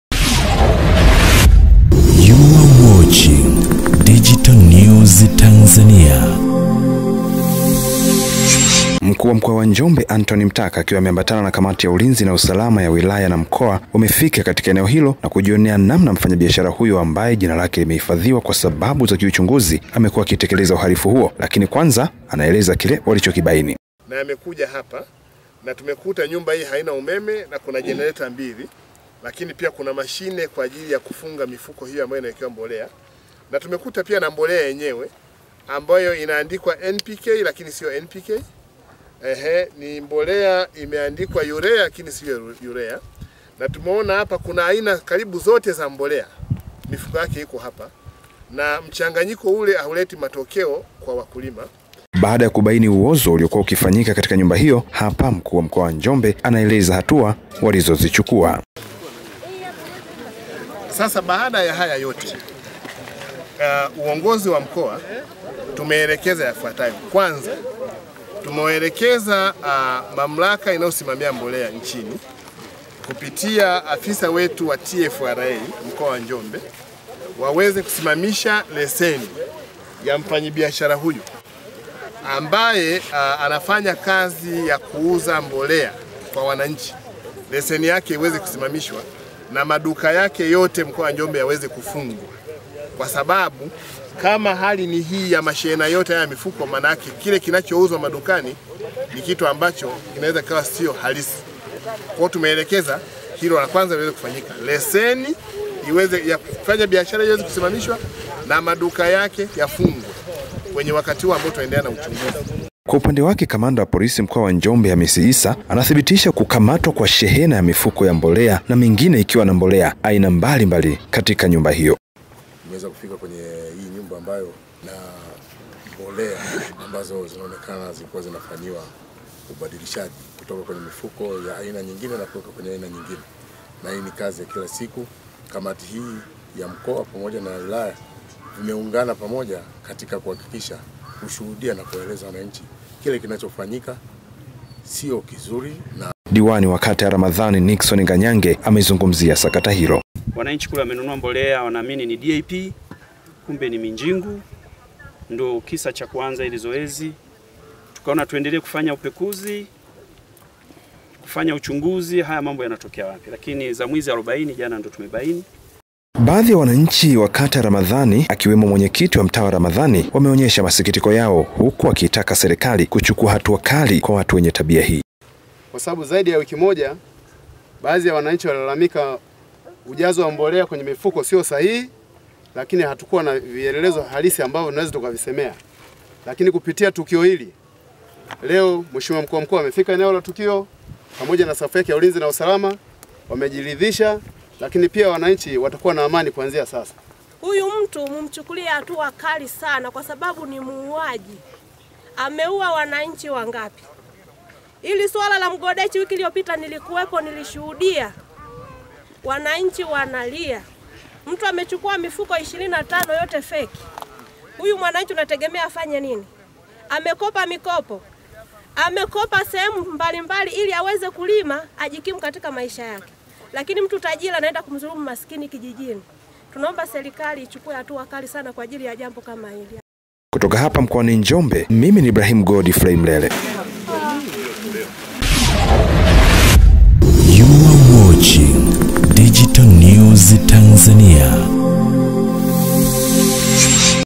NYUMBA INAYOTUMIKA KUTENGEZA MBOLEA FEKI YAPATIKANA RAMAZANI NJOMBE/RC MTAKA ATOA TAMKO/
Serikali Mkoani Njombe imefanikiwa kunasa Nyumba Moja iliyopo katika Mtaa wa Ramazani kata ya Ramazani Halmashauri ya Mji wa Njombe ambayo imekuwa ikitumika Kuchakachua Mbolea za Ruzuku na Kisha Kuziuza kwa Wakulima katika Maeneo mbalimbali ya Mkoani Njombe.